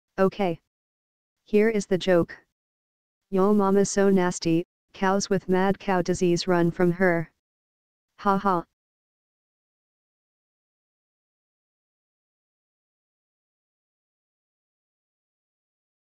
Ok, here is the joke. yo mama so nasty... cows with mad cow di...
yo mama so nasty... cows with mad cow disease run from her..